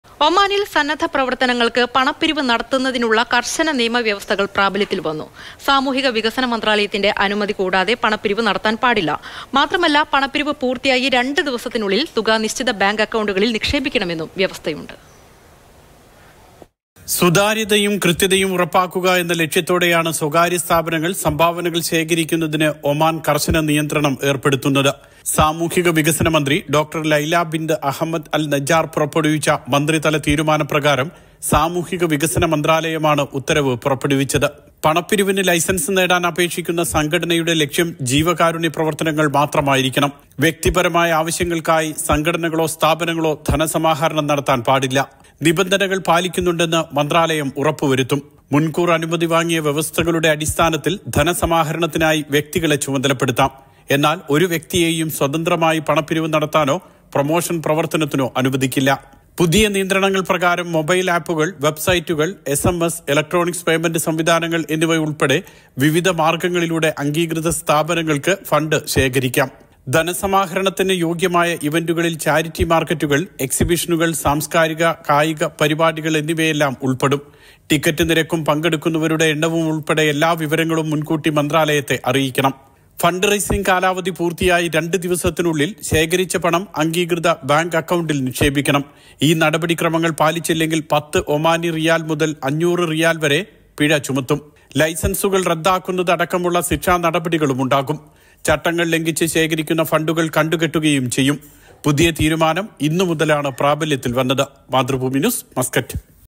ഒമാനിൽ പണപ്പിരിവ് നടത്തുന്നതിന് കർശന നിയന്ത്രണം; പണപ്പിരിവിന് അധികൃതരുടെ അനുവാദം വേണം
മാറുന്ന ലോകത്ത് കൂടുതൽ മാറ്റങ്ങളോടെ. പുതിയ വേഗത്തിൽ. പുതിയ ലുക്കിൽ.
മാതൃഭൂമി ന്യൂസ്.

Happy viewing!